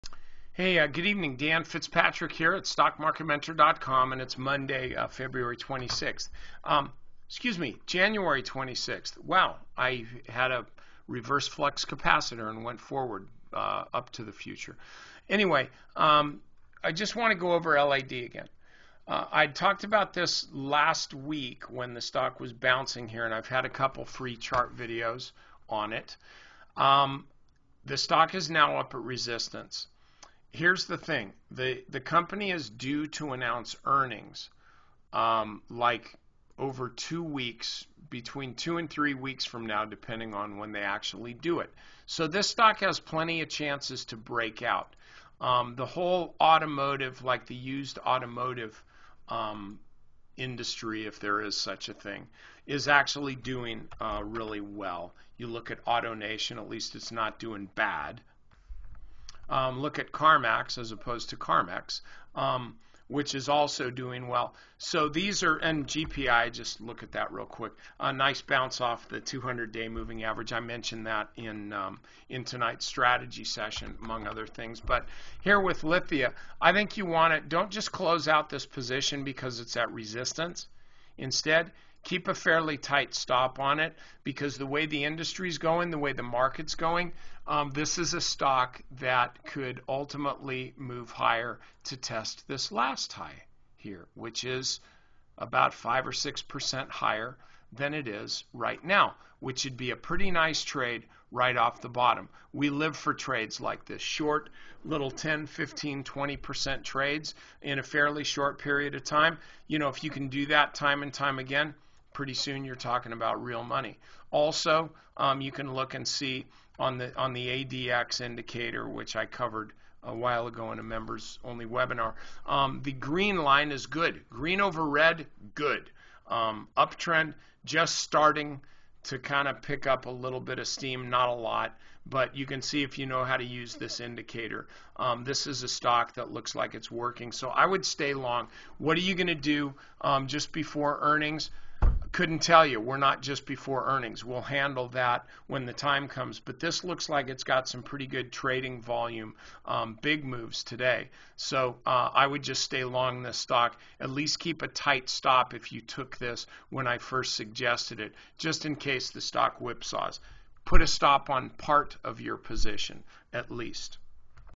Sell when your trade hits resistance? Not so fast! Here's another look at Lithia Motors (LAD) (..
Sell when your trade hits resistance?  Not so fast!  Here's another look at Lithia Motors (LAD) Stock Market Mentor